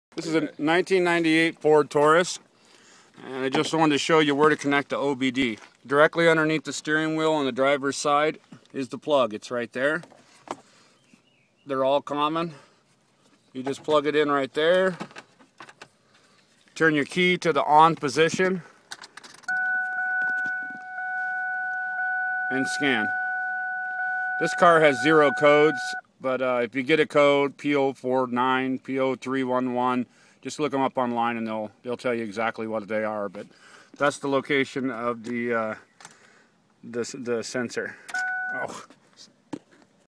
ODB II location / connector on a 1998 Ford Taurus
Here I am showing were the ODB II connector is on a 1998 Ford Taurus. Ever scanner tool is different but the connection is the same.